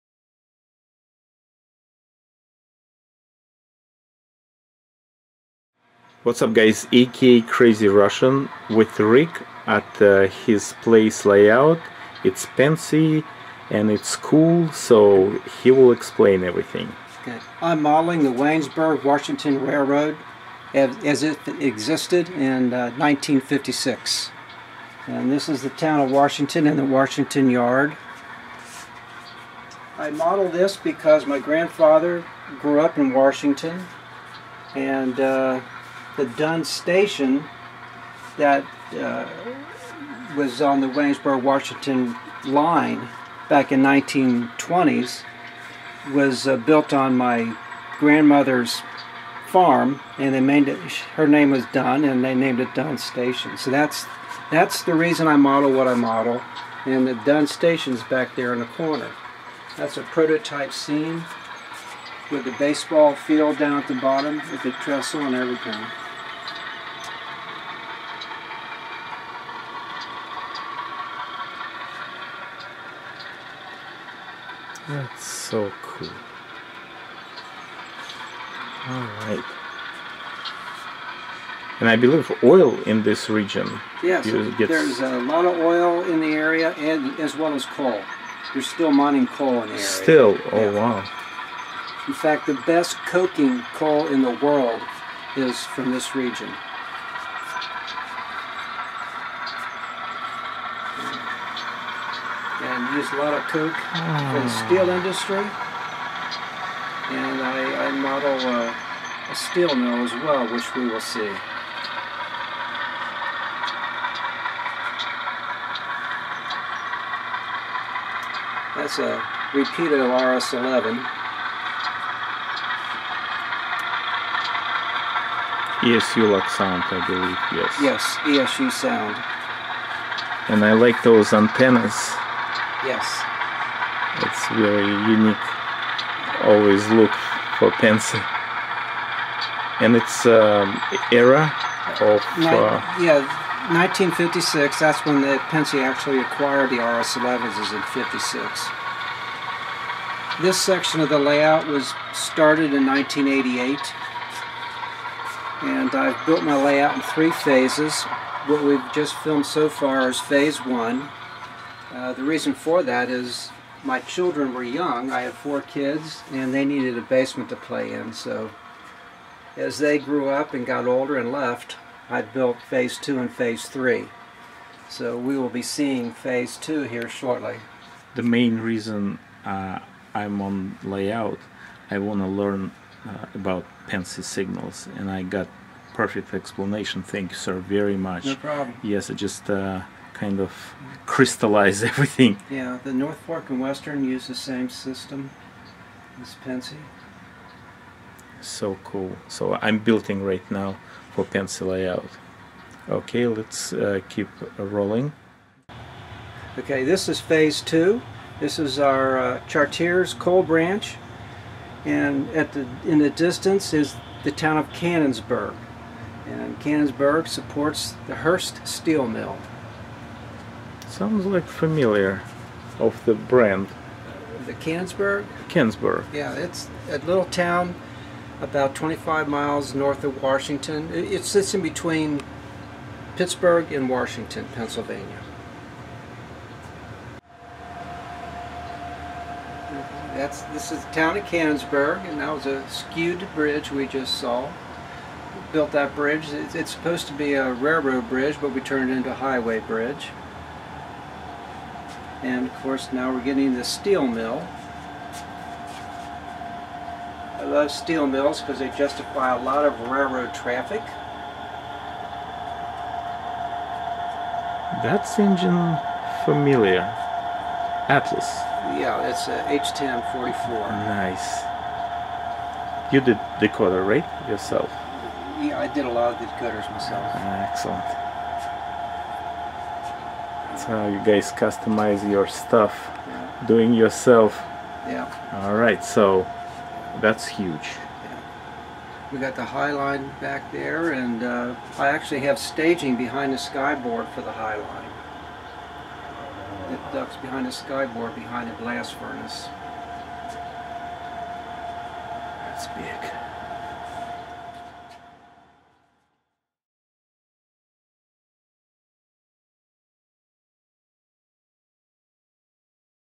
Waynesburg and Washington Railroad HO Layout Part 1
The Waynesburg and Washington Railroad was a 28-mile 3-foot gauge subsidiary of the Pennsylvania Railroad. From the 1870s through the 1920s the line served its namesake towns in Southwestern Pennsylvania. After the 1930s, the line did struggle on, but mostly on paper.
Locale: Pittsburgh Metropolitan Area
Founded: 1868